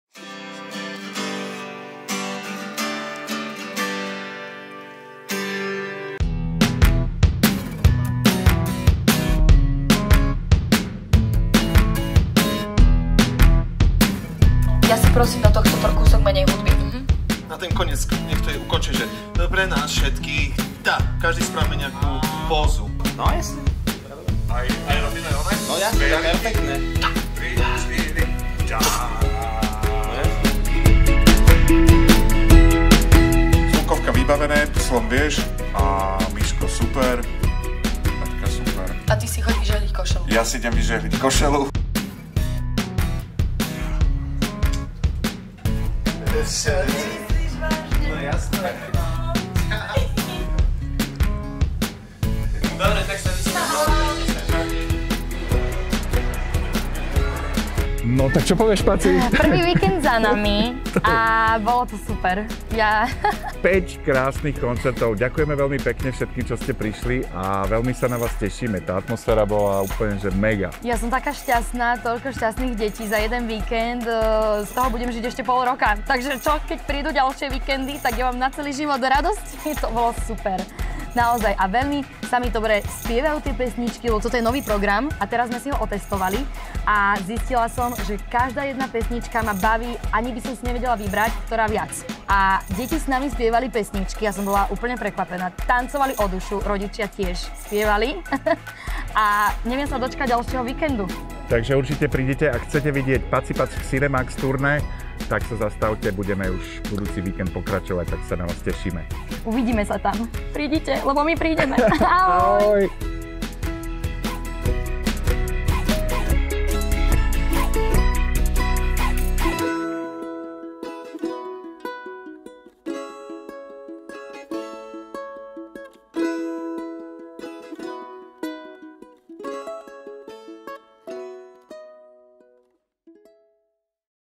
PACI PAC v CINEMAX II. / DOKUMENT
Krátky dokument z prvého víkendu turné PACI PAC v CINEMAX II.

Kamera/strih: Majo Puzder